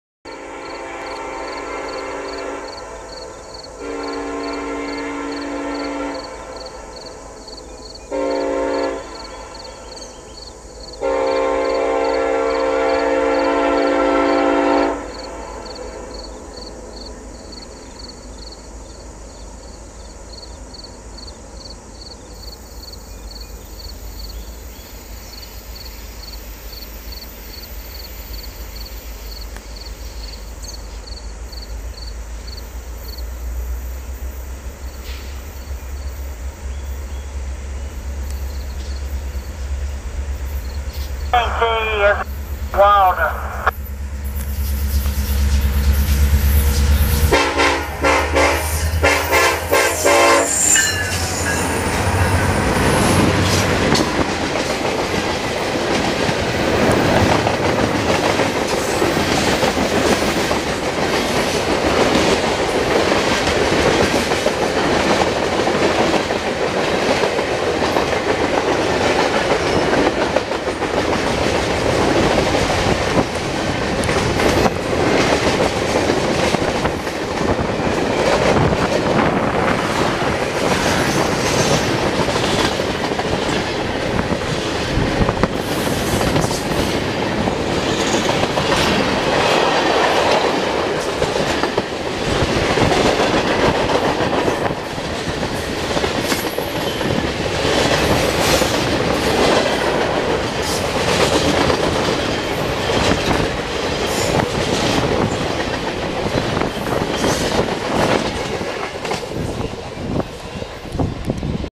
Northbound 16T At Wilda Signal Engineer Waves Gives Horn Salute
Sunday August,14,2016 northbound N/S 16T Engineer calls clear at Wilda Signal, waves and gives horn salute